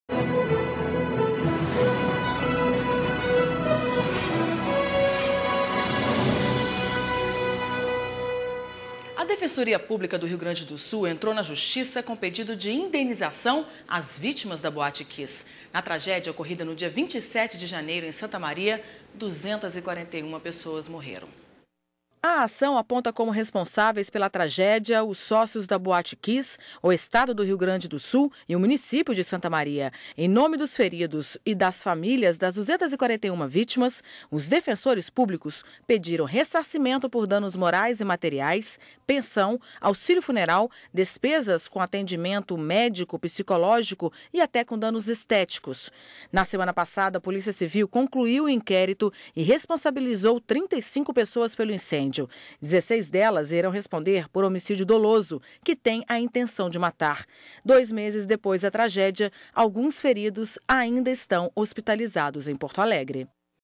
Indenização boate Kiss (28/03/13)
Defensoria Pública do Rio Grande do Sul entra na Justiça com pedido de indenização às vítimas da boate Kiss. Na tragédia, ocorrida no dia 27 de janeiro, em Santa Maria (RS), 241 pessoas morreram.